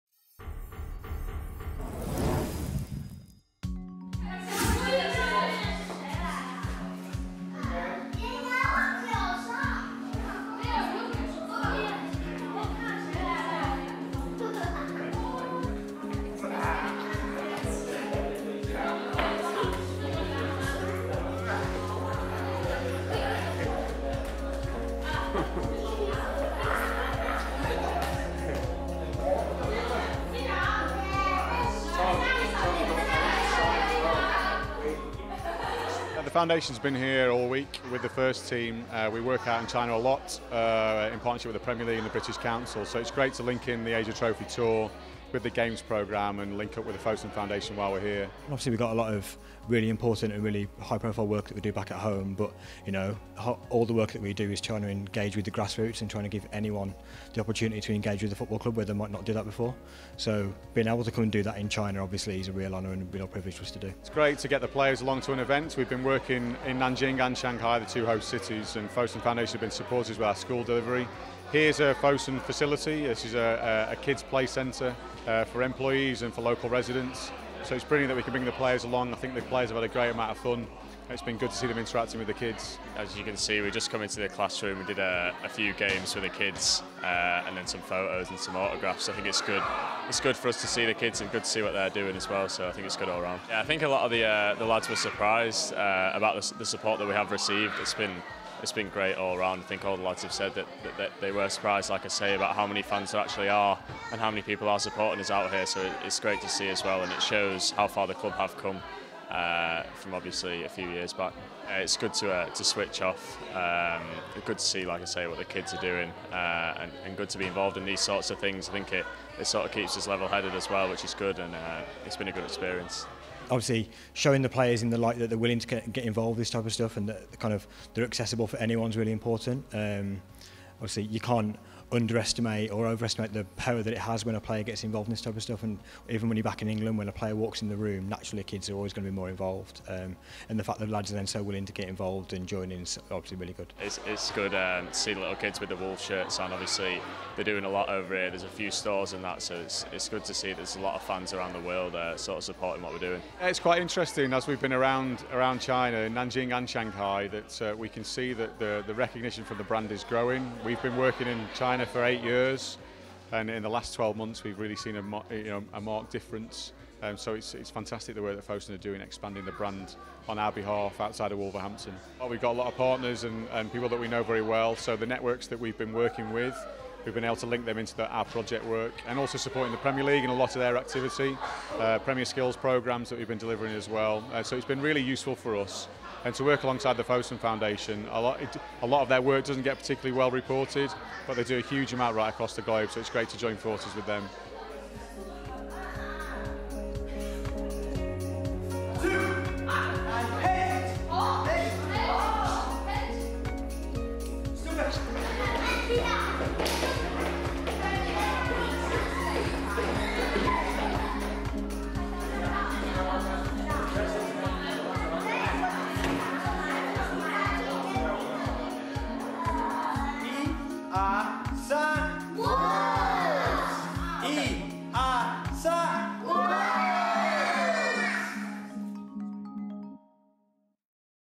Wolves Foundation in China | Gibbs-White, Ed Francis and Christian Marques visit Fosun Miniversity
Wolves Foundation have been out in the community in Shanghai, laying on sessions for youngsters with the help of a trio of Wolves players. Morgan Gibbs-White, Ed Francis and Christian Marques attended Fosun's Miniversity to help Foundation staff, who laid on fun and games for children aged three to six.